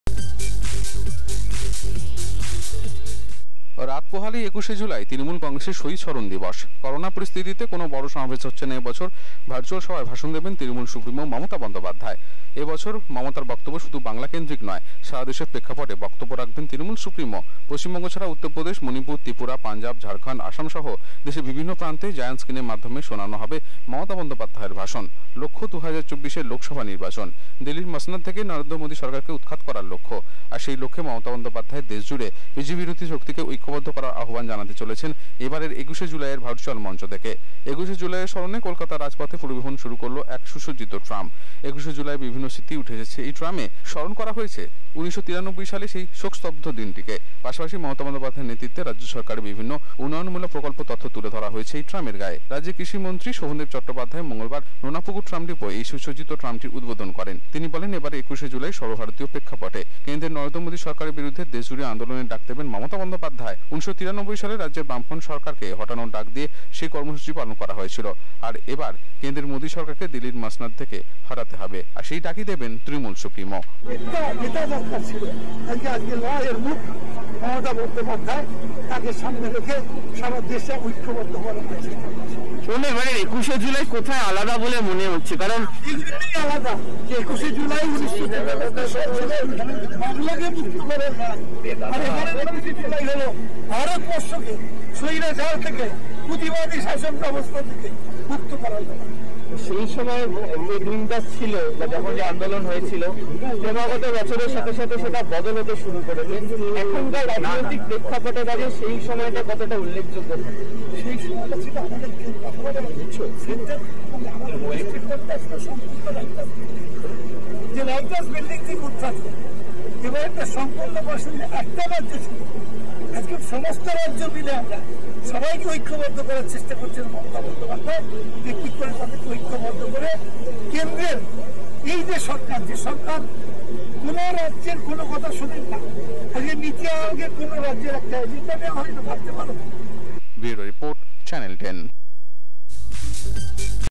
২১ শে জুলাইয়ের স্মরণে কলকাতার রাজপথে সুসজ্জিত ট্রাম
২১ শে জুলাইয়ের স্মরণে কলকাতার রাজপথে সুসজ্জিত ট্রাম